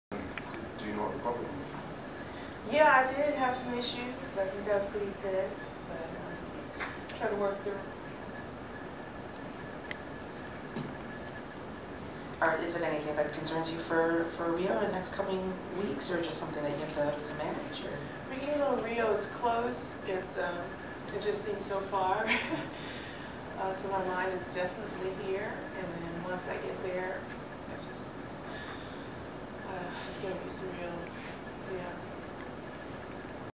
Venus Williams - has some issues
The American star avoids the specifics in discussing a very evident problem on serve during her Wednesday match in Montreal against Barbora Strycova.
For more tennis, go to opencourt.ca